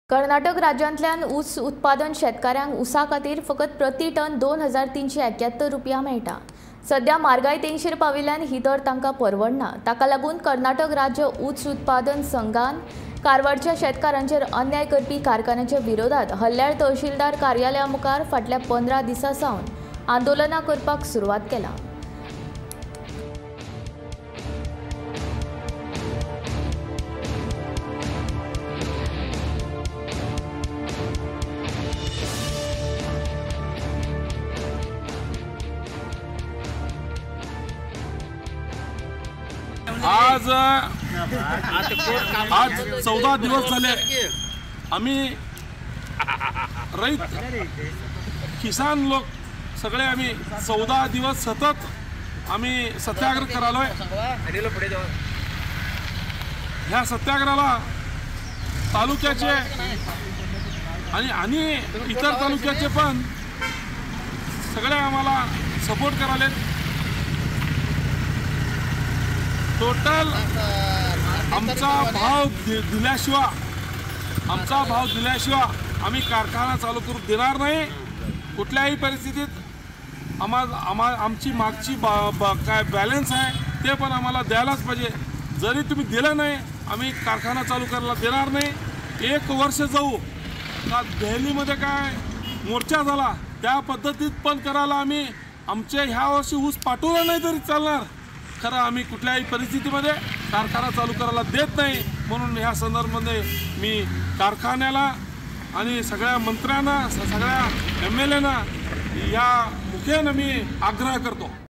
Karwar district sugarcane farmers demand increase in FRP | Gomantak TV
कारवार जिल्ह्यातील ऊस शेतकऱ्यांची एफआरपी वाढवण्याची मागणी | Karwar district sugarcane farmers demand increase in FRP | Gomantak TV

Karawar District farmers protest | Sugarcane farmers | Increase in FRP